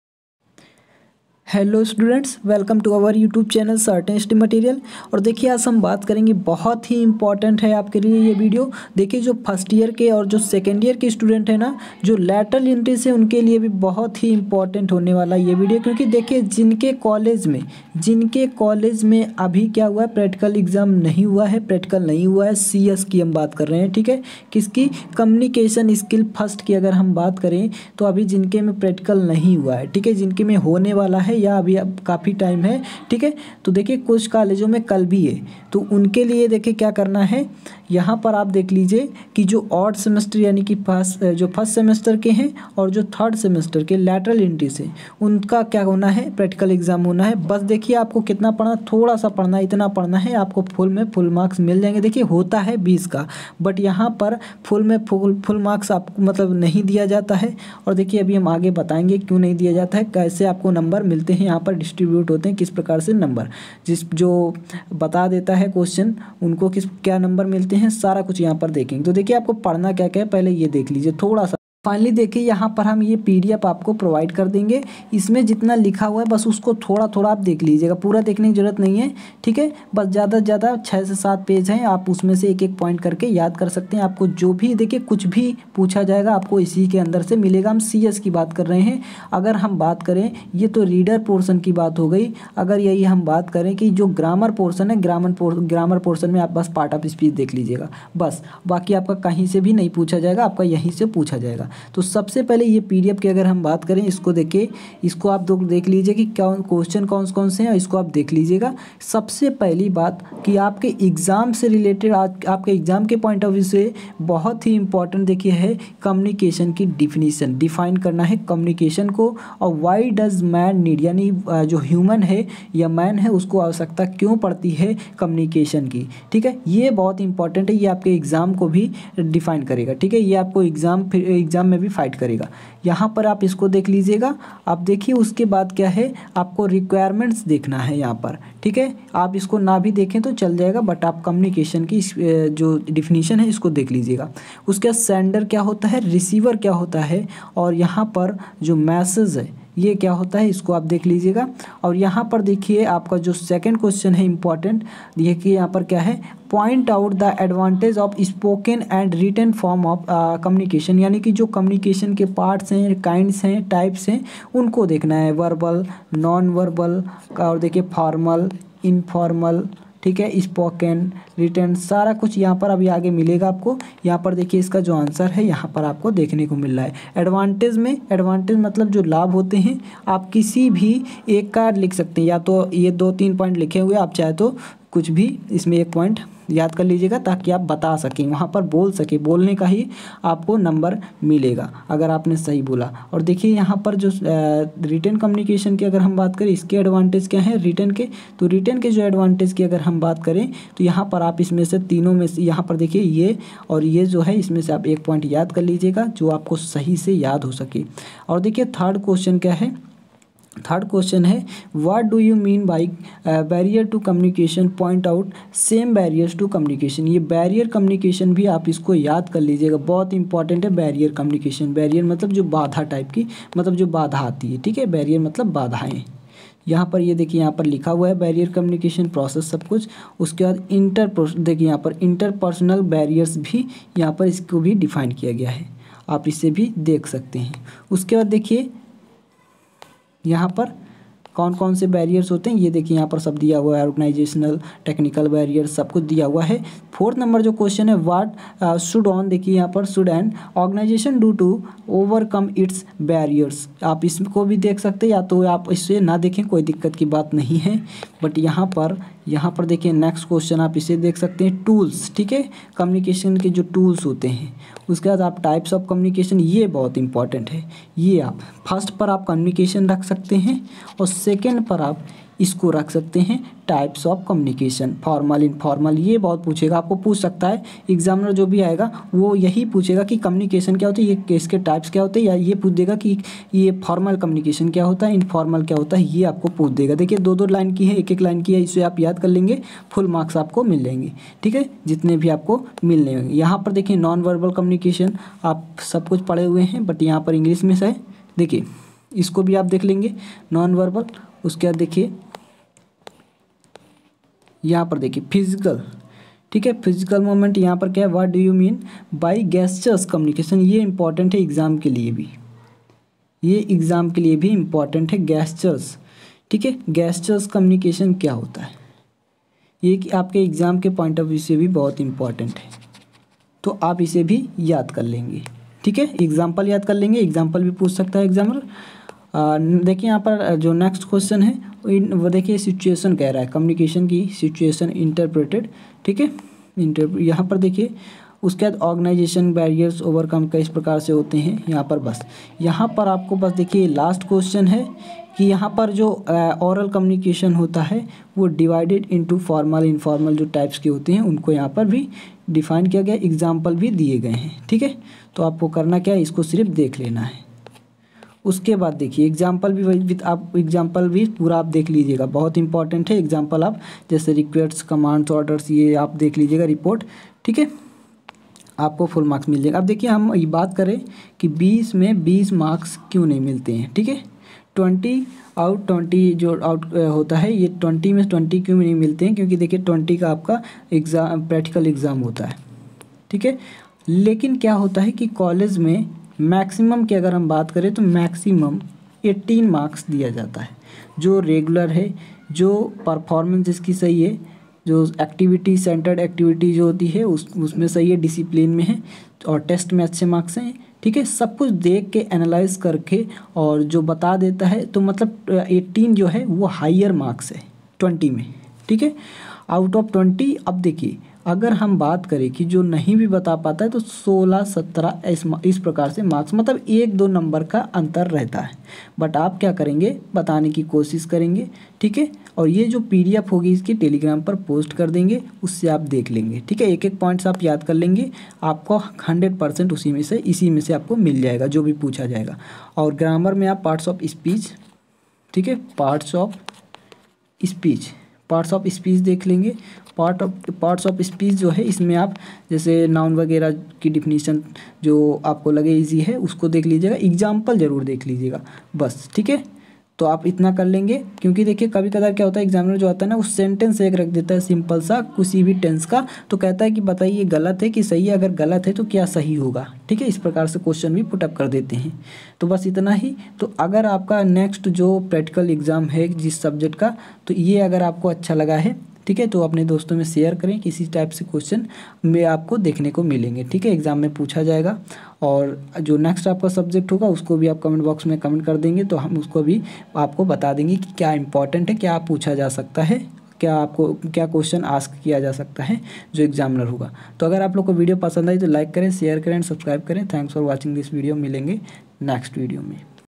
Important questions || Practical exam || Communication skill - 1st.
Polytechnic 1st Semester & 3rd semester Lateral entry ke Communication skill - 1st
ke Practical exam ke liye most important
and selected questions.